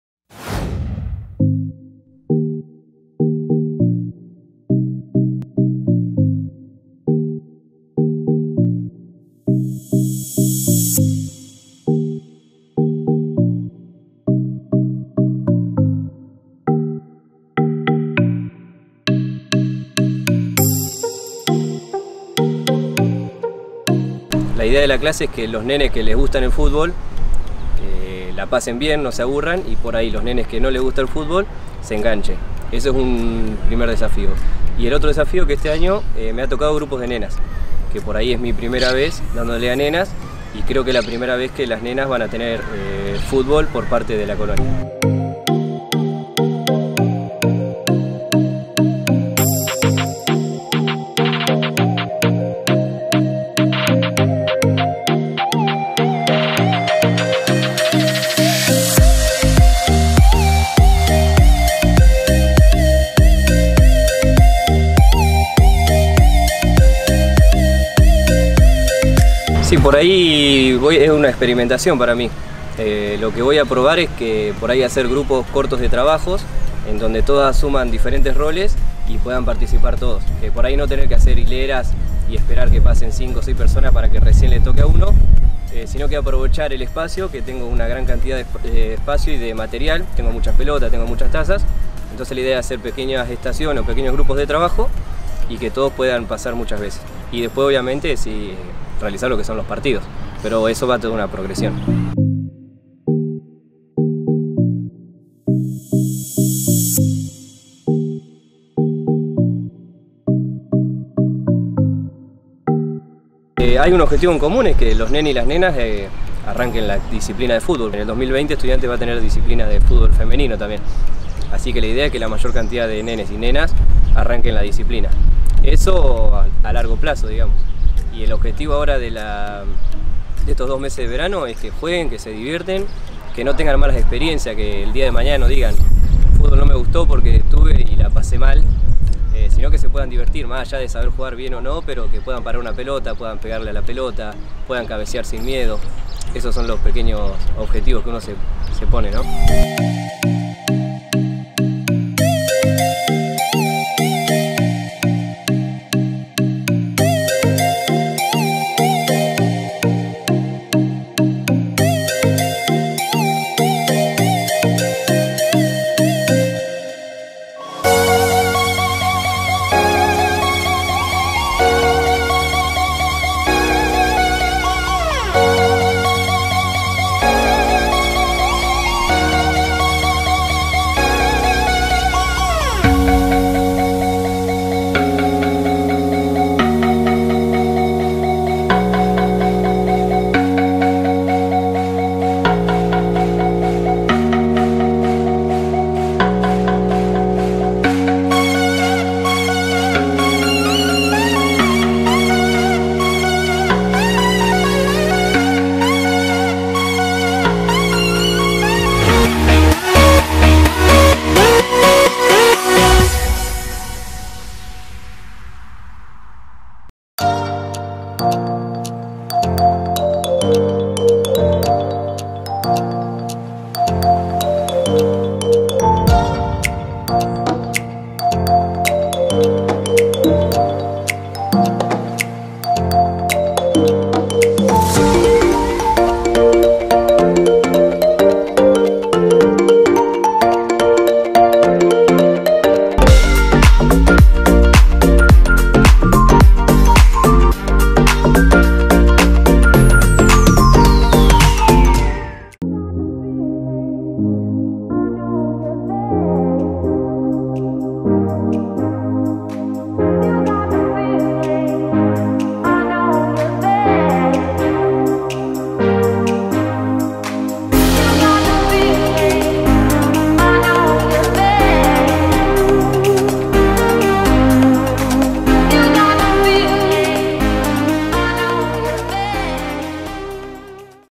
Fútbol de colonia
En el Club de Niños de Estudiantes los niños y niñas conocen todos los deportes que se practican en el club durante el año. En este video, el profesor Angel Bosaleh cuenta cómo se les enseña a los más chiquitos los primeros pasos en el fútbol.